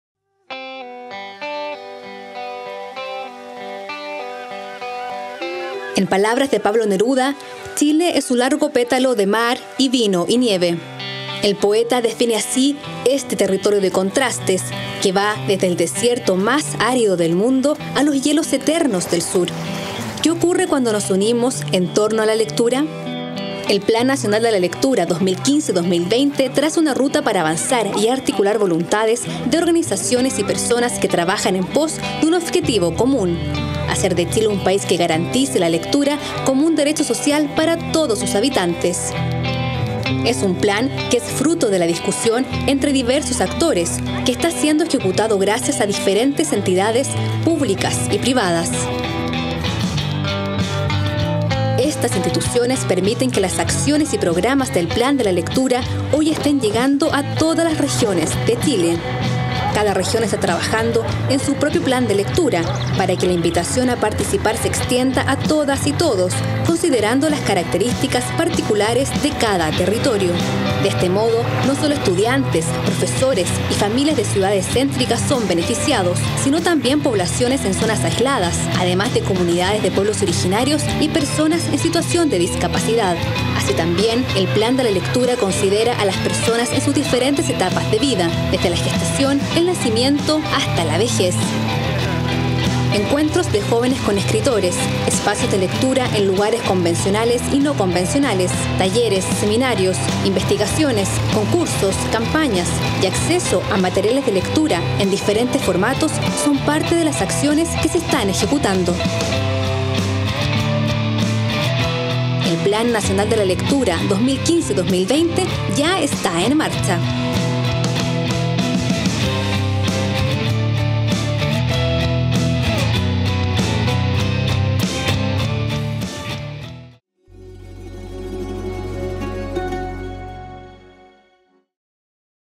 Plan Nacional de la Lectura 2015 - 2020
Plan Nacional de la Lectura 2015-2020 reconoce a la lectura como un derecho social para todos los habitantes de Chile, donde los siguientes organismos del Estado se articulan para la formulación de acciones en conjunto: Ministerio de Educación (Mineduc) y Junta Nacional de Auxilio Escolar y Becas (Junaeb); Consejo Nacional de la Cultura y las Artes (CNCA); Dirección de Bibliotecas, Archivos y Museos (Dibam); Ministerio de Desarrollo Social (MDS), a través del Sistema de Protección Integral a la Infancia Chile Crece Contigo y sus servicios asociados Servicio Nacional del Adulto Mayor (Senama), el Instituto Nacional de la Juventud (Injuv), el Servicio Nacional de la Discapacidad (Senadis), el Fondo de Solidaridad e Inversión Social (Fosis), y el Ministerio Secretaría General de la Presidencia (Minsegpres), por medio del Consejo Nacional de la Infancia.